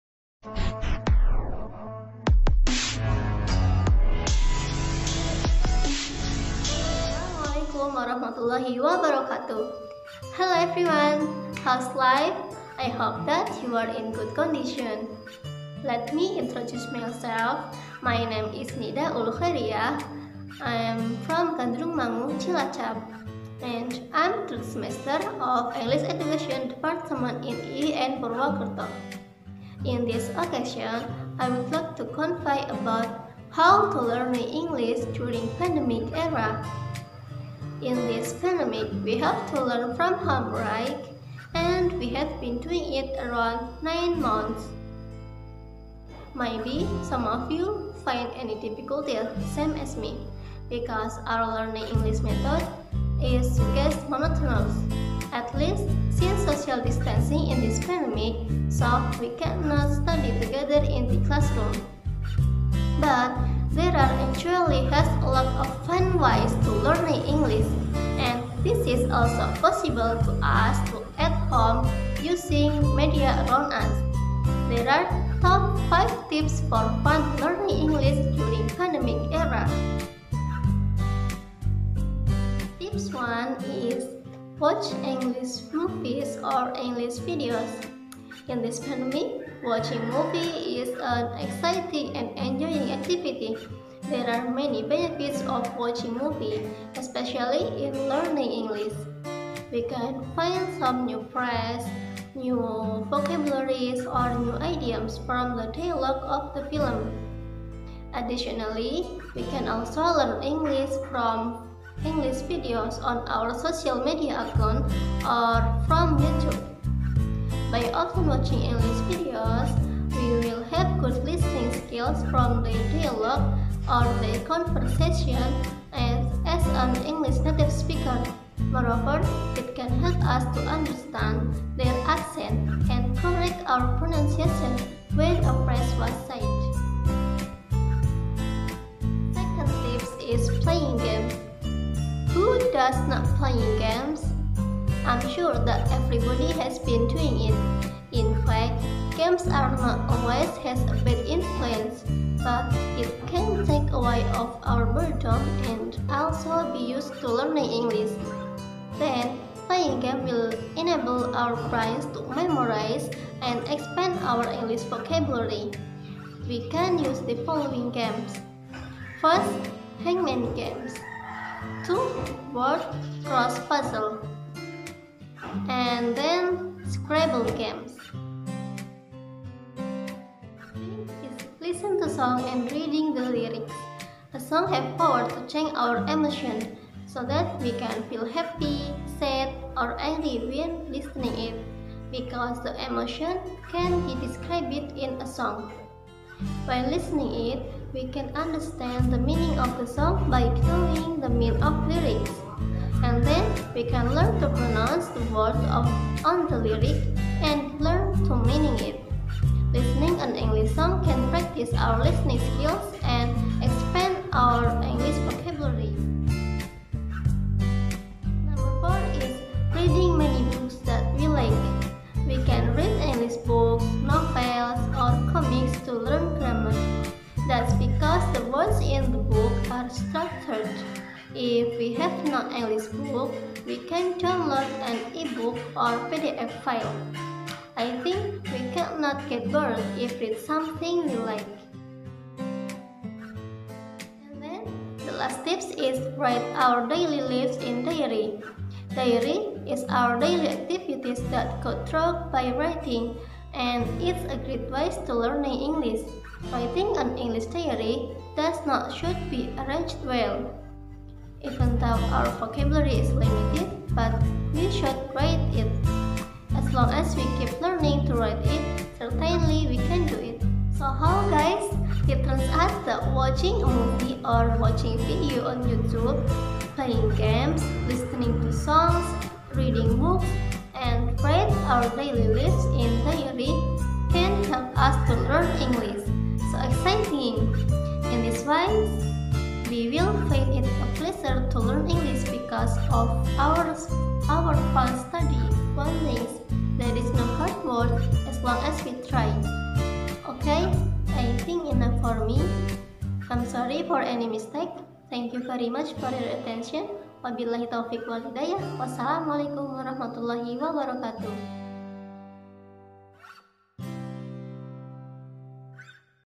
How to Learn English During Pandemic Era??
Speaking for Academic Purposes

Name : Nida'ul Khoeriyah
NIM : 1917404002
Class : 3 TBI A
Topic : How to Learn English During Pandemic Era

I'm sorry for any mistakes..